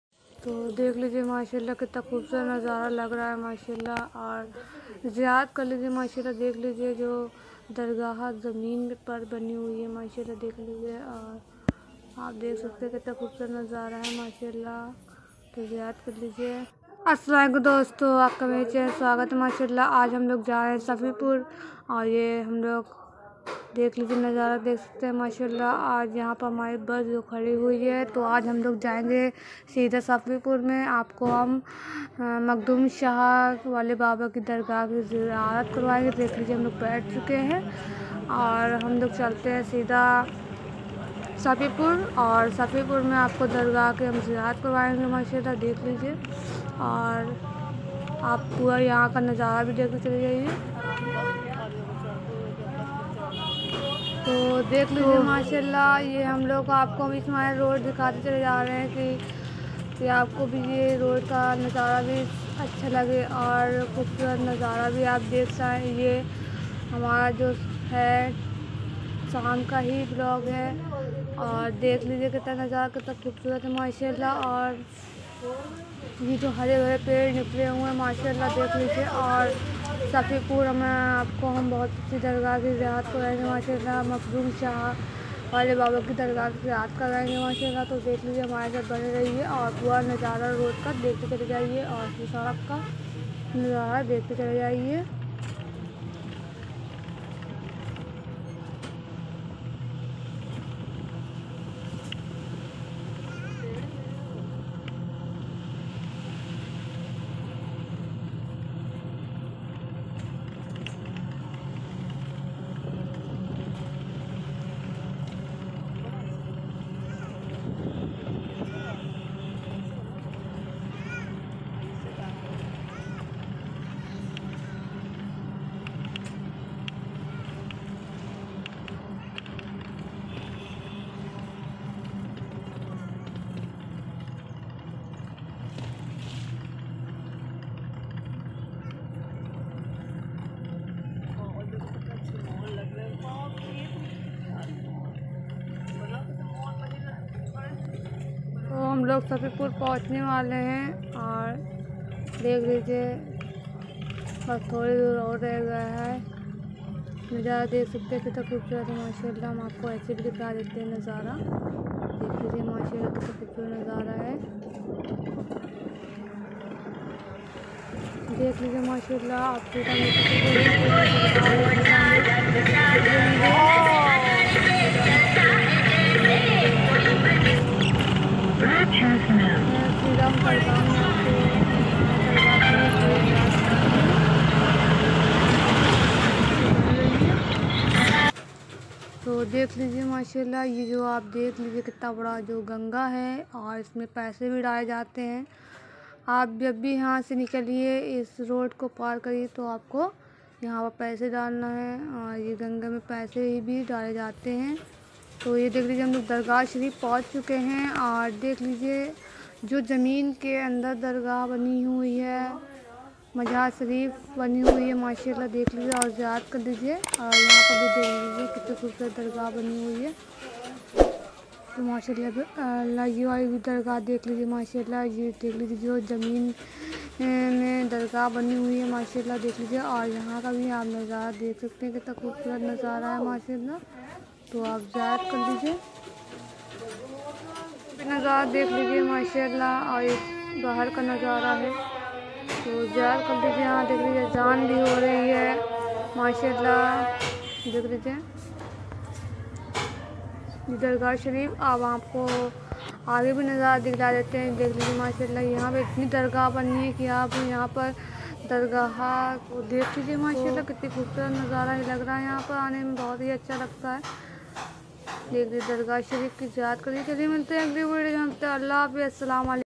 Shafipur dargah Makhdoom shah Wale Baba Ki Dargah Ki Ziharat 2023 Aur Beautiful Mahol 2023
Dosto Es Video Me Mene Shafipur Dargah Makhdoom Shah Wale Baba Ki Dargah Ki Ziharat Karwayi Hain Aur Khubsurat Mahol Dikhaya Hain Yaha Khubsurat Nazara Bhi Dikhaya Hain Yaha Dur Se Long Ate Hai Apni Manki Murade Lekar Aur Yaha Dargah Shafipur Me Yaha Dargah Padti Hain

My Channel Name is Rasoi Ghar And Vlog Hain

Mere Channel Ko Support Karo
Like Karo
Share Karo